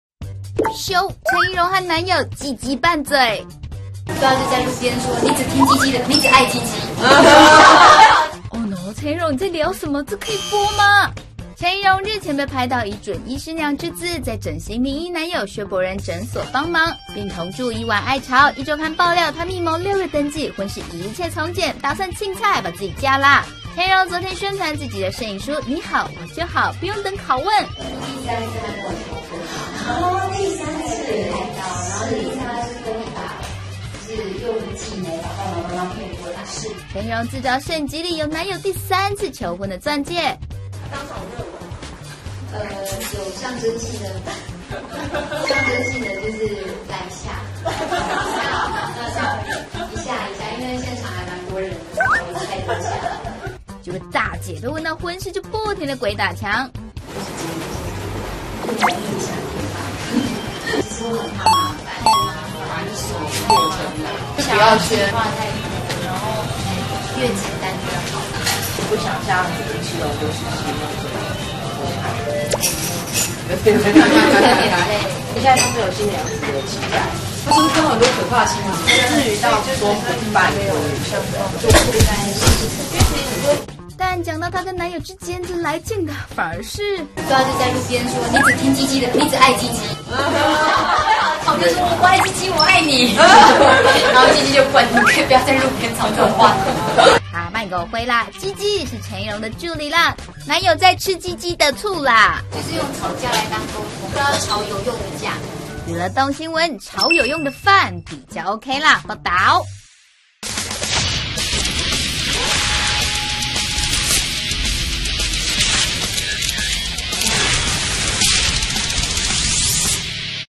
陳怡蓉男友黃腔收服準岳母 婚禮無期「有了就生」--蘋果日報20160319
【葉婉如╱台北報導】37歲的陳怡蓉日前被爆已和整形診所CEO薛博仁甜蜜入住億萬愛巢，且婚期就在今年夏天，昨她出席攝影書《我好，你就好》發表會，是否已登記結婚？她靜默後起身尿遁，回座時才說：「有好消息會跟大家分享，又不是什麼見不得人的事，你們怎麼比我爸媽還急！」至於「做人」，她表示：「若有了就會生。」